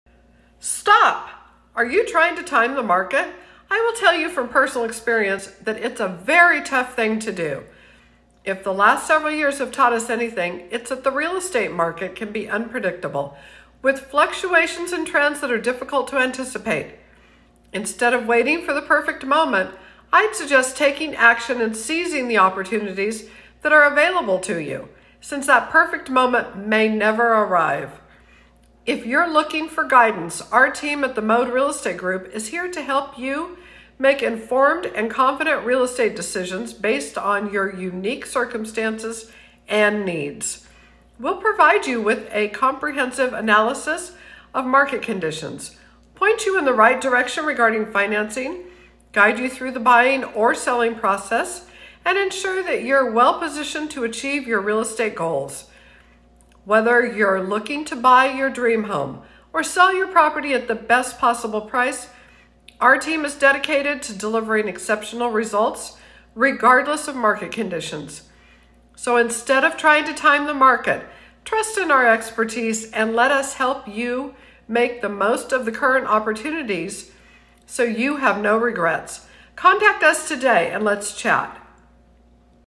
Stop trying to time the market!
Stop trying to time the market!  Quit waiting for the perfect moment!  Listen to what you should do instead!